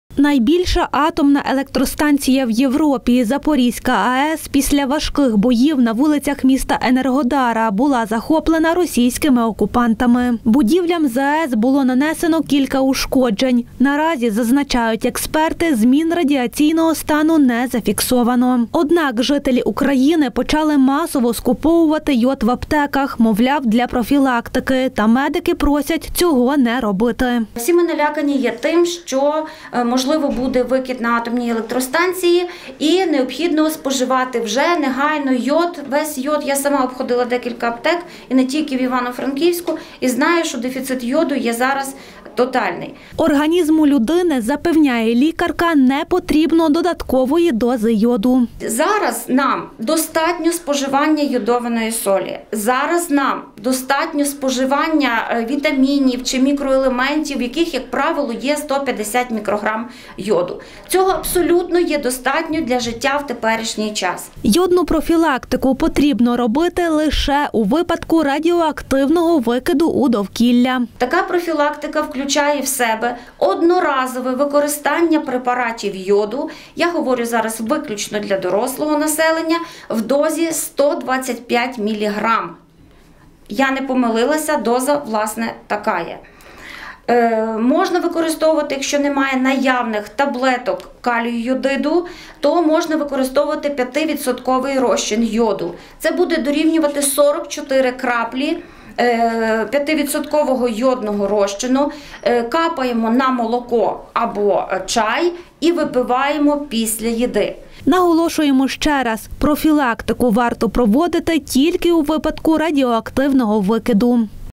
Після обстрілу окупантами ЗАЕС в України різко зріс попит на препарати з йодом
Ажіотаж на йод. Після обстрілу окупантами Запорізької атомної електростанції в України різко зріс попит на препарати з йодом. Українці фактично викупили весь такий товар. Немає йоду і в Івано-Франківську. Що кажуть медики – дивіться далі.